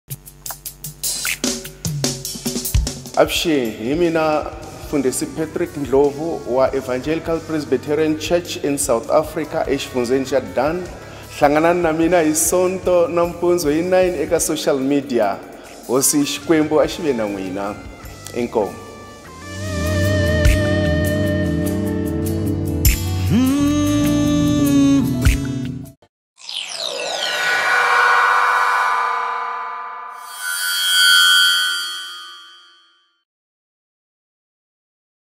Rev P Ndlovu Advert
Rev P Ndlovu from Dan EPC SA Parish invites you for a sunday ONLINE DIGITAL service on Facebook and also on YouTube. Sunday 29 March 2020